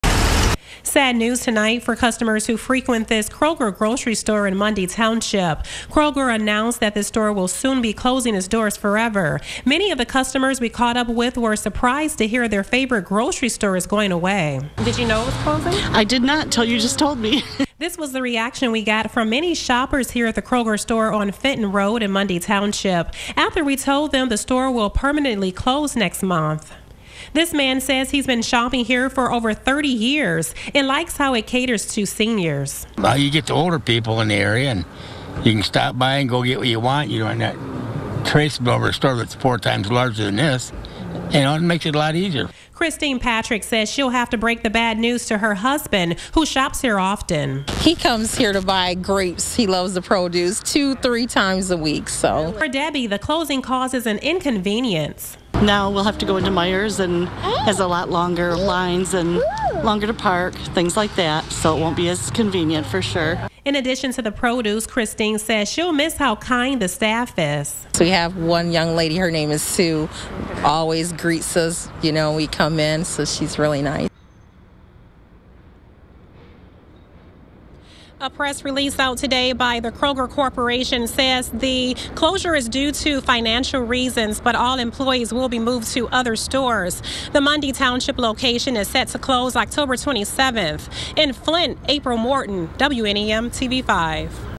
Kroger closing store in Genesee County
Kroger announced it is closing its store in Mundy Township.